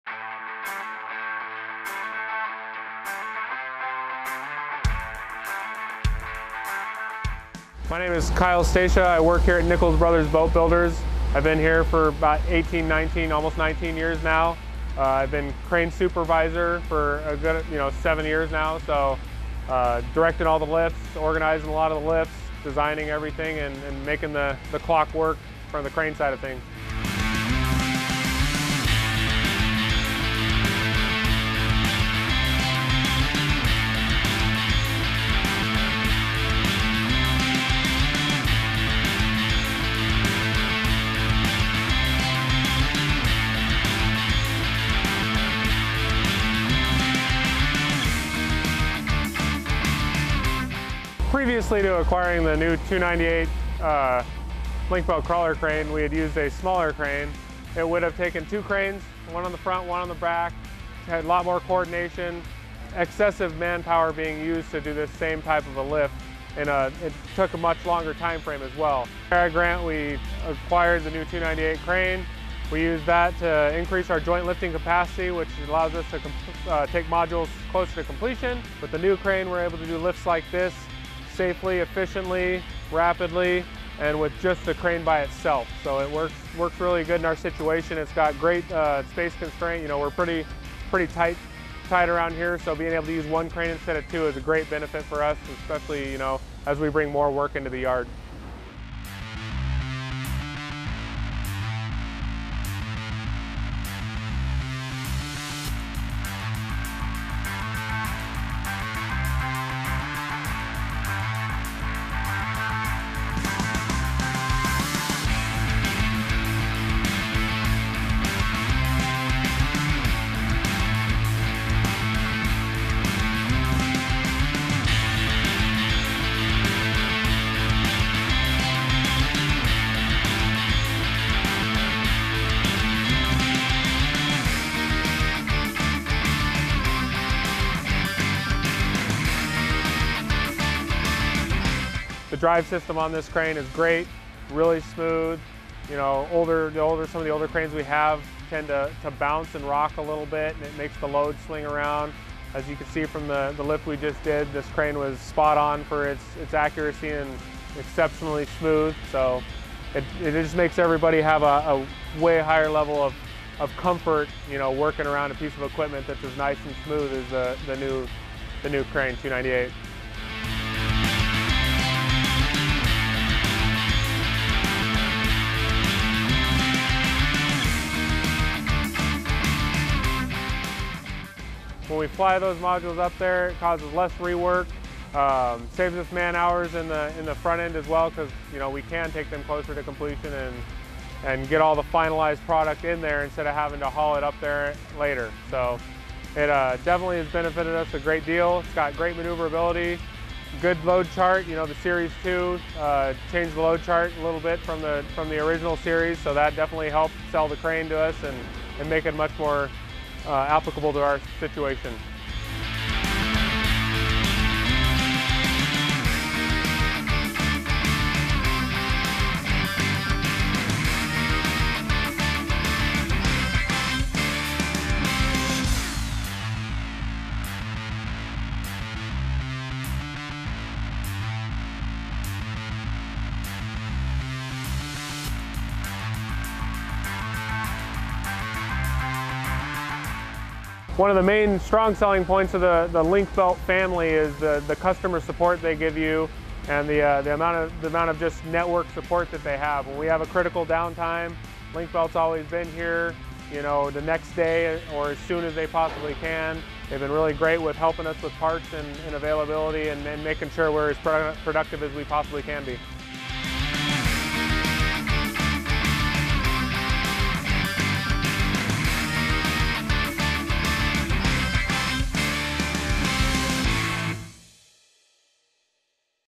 298 Series 2 Shipbuilding in Washington
Nichols Brothers Boat Builders of Freeland, Washington purchase of a Link-Belt 250-ton (227-mt) 298 Series 2 lattice crawler crane for shipbuilding construction at their location on Whidbey Island in Puget Sound.